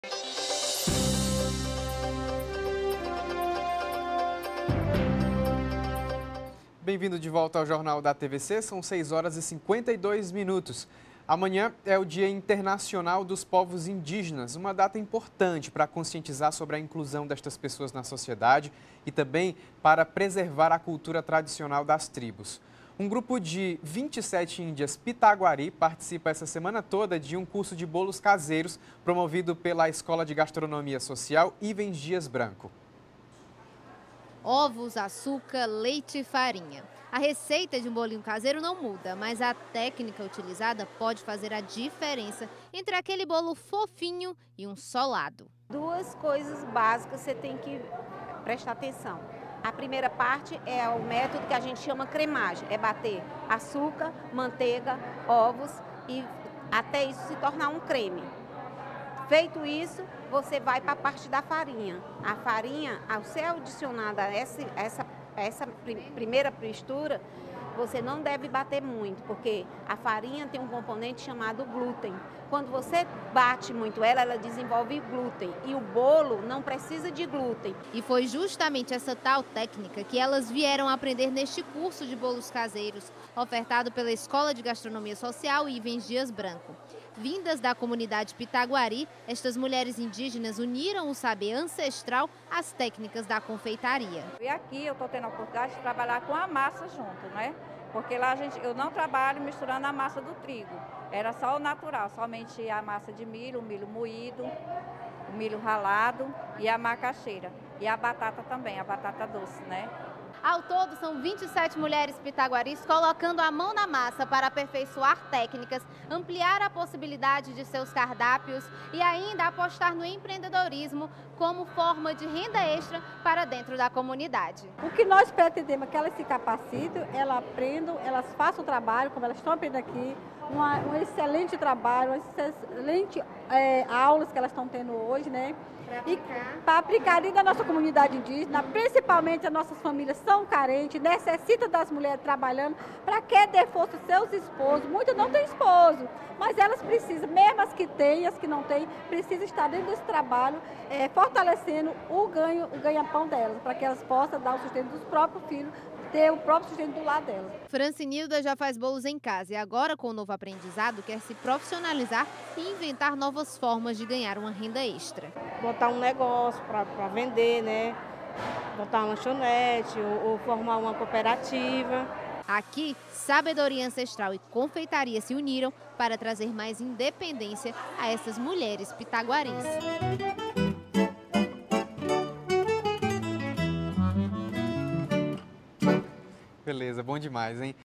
Índias Pitaguary participam de curso gastronômico. | Jornal da TVC 08.08.19
Escola de Gastronomia Ivens Dias Branco trabalha a inclusão social por meio de  curso de bolos caseiros. Em celebração ao Dia Internacional dos Povos Indígenas, 27 índias da tribo Pitaguary participam do curso durante uma semana. Entenda.